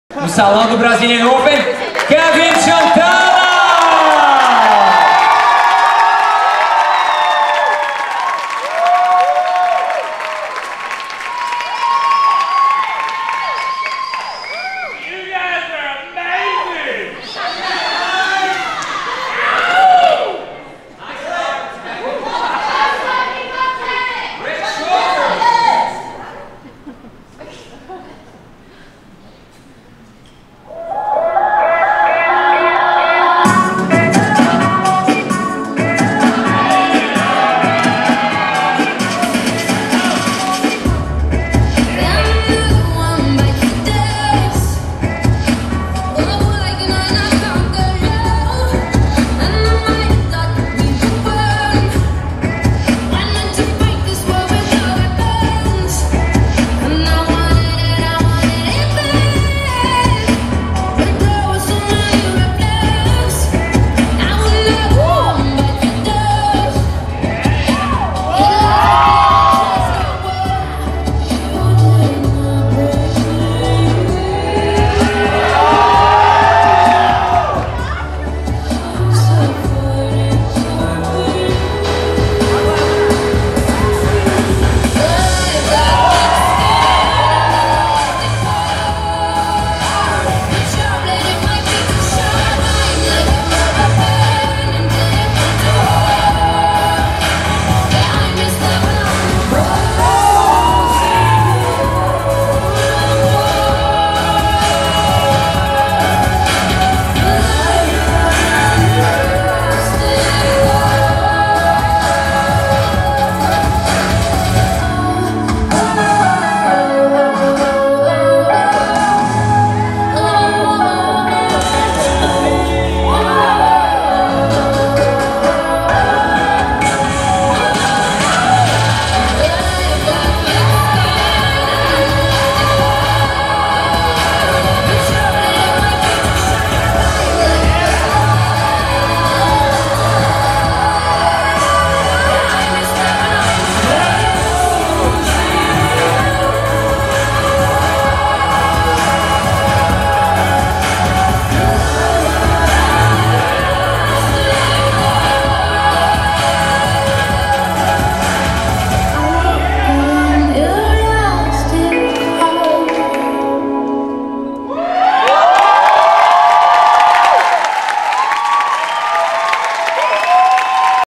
2016 Brazilian Open Championships Routine - Kevin Kane and Shantala Davis
2016 Brazilian Open Championships Routine. Performed by Kevin Kane (USA) and Shantala Davis (USA).